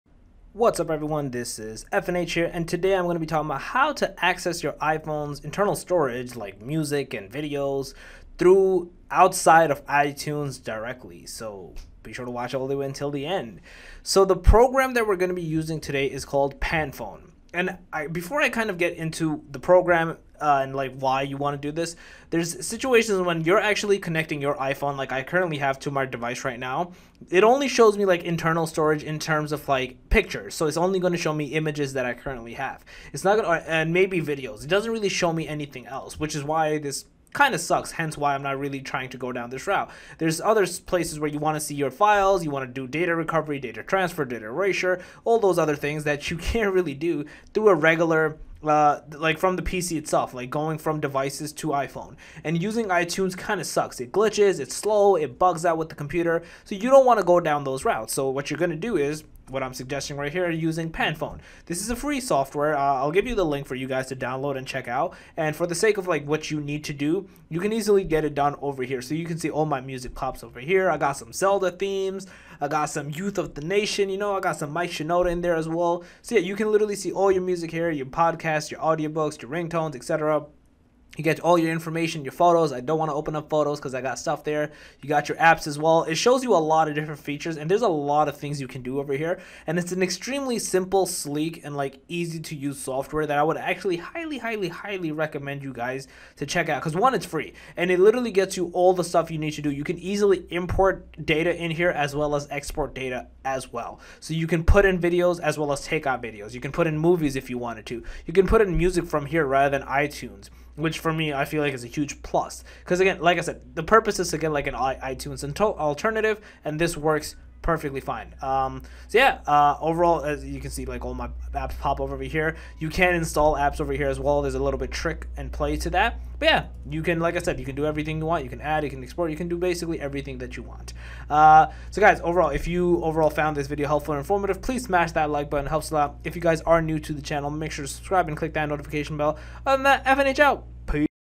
Best Free iTunes Alternative! Panfone Tutorial 2020
Hey All! in this video i talk about Best iTunes Alternative! Panfone Tutorial